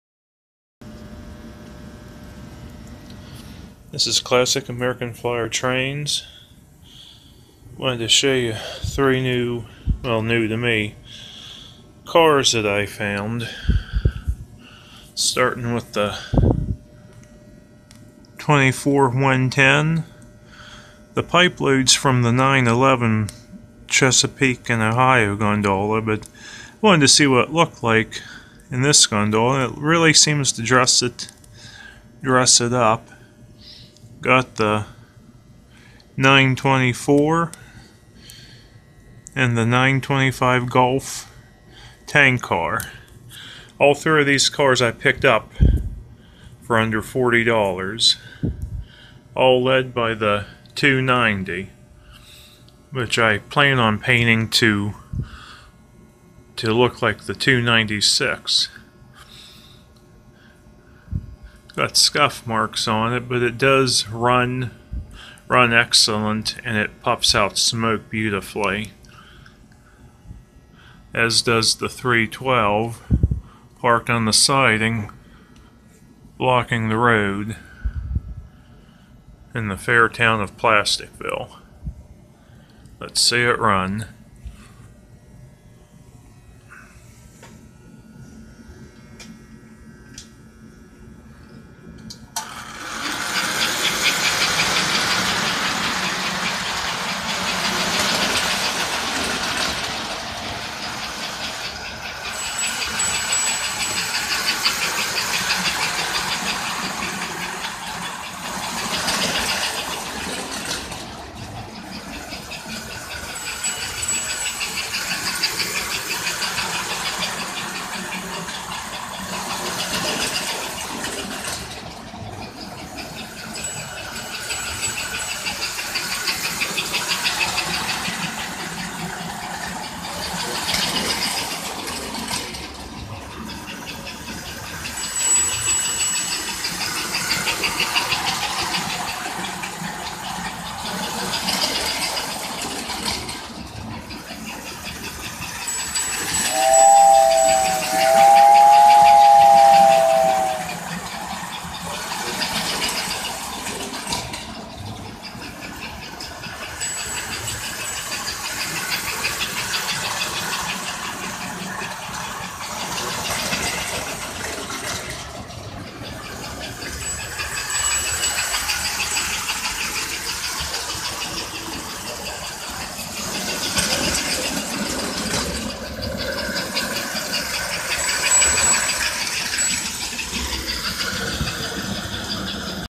Three new American flyer cars!
Three new pieces of rolling stock for under $40! The even though the 24110 gondola has pikemaster fixed knuckle couplers I really like the look of it.